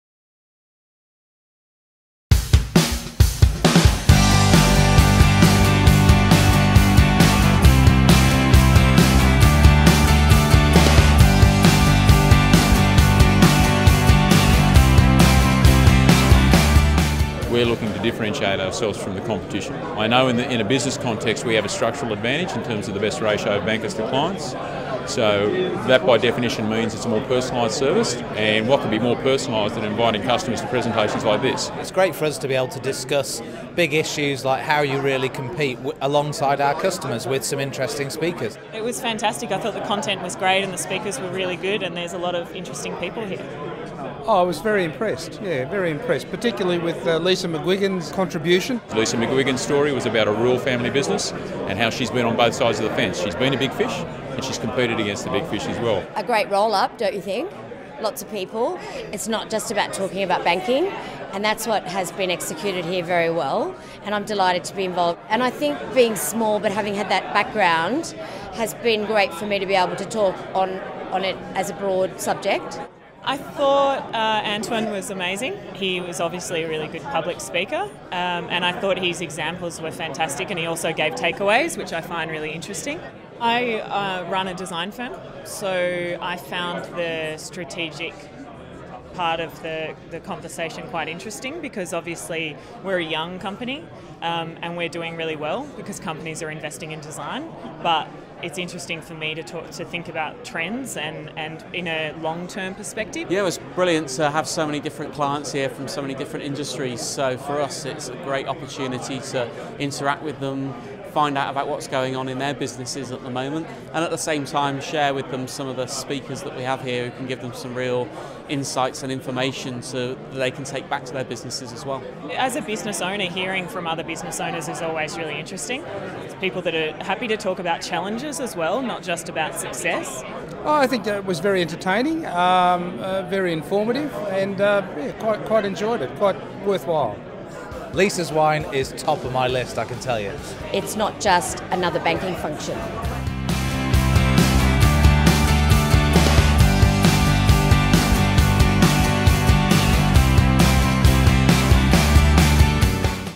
Bankwest Connect Series events - ideas, insights & innovations for business leaders
At Bankwest we believe that a modern business bank should provide support for every aspect of your operations, helping to give your business the edge. Since 2010 Bankwest Connect Series has provided business leaders with ideas, insights and innovation that have helped them to address the challenges they face and, ultimately, grow their organisations.

Bankwest Connect events:

• Range from keynote events to smaller, more intimate, roundtable discussions
• Are a valuable opportunity to get exclusive access to content and industry experts
• Provide a great chance to network and unwind with industry peers
• Are open to any business, not just Bankwest customers